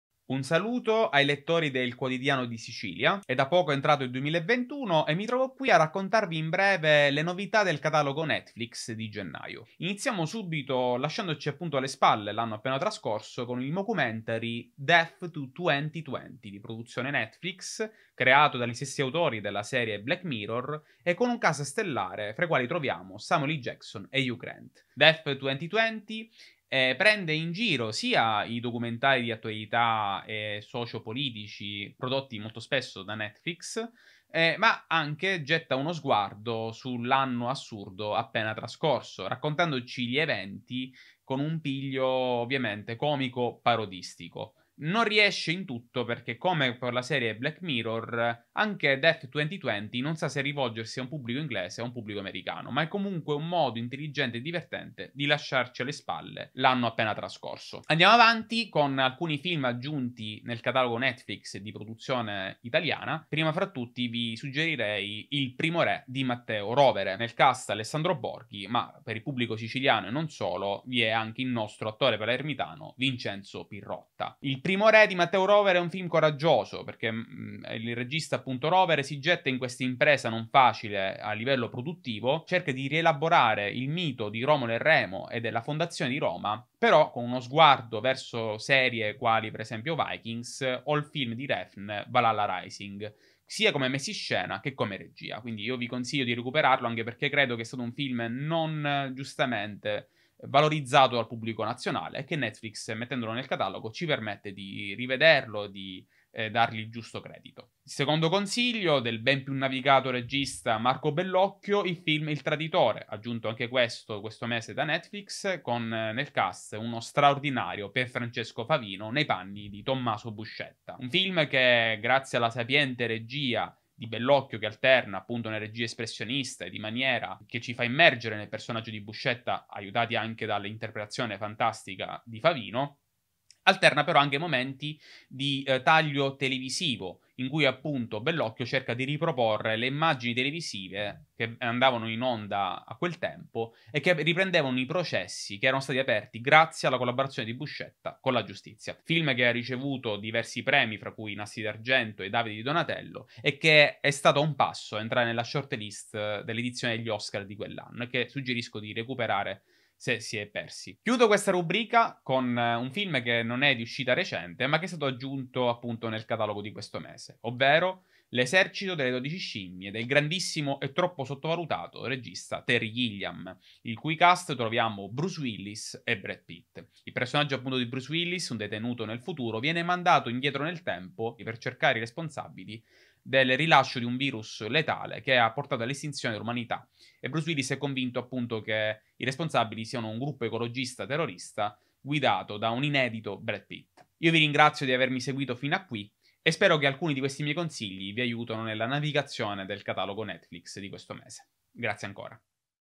Cosa guardare su Netflix a gennaio, i consigli del regista Bruno Mirabella
I consigli del Regista Bruno Mirabella: serie tv e film. Ecco quali guardare.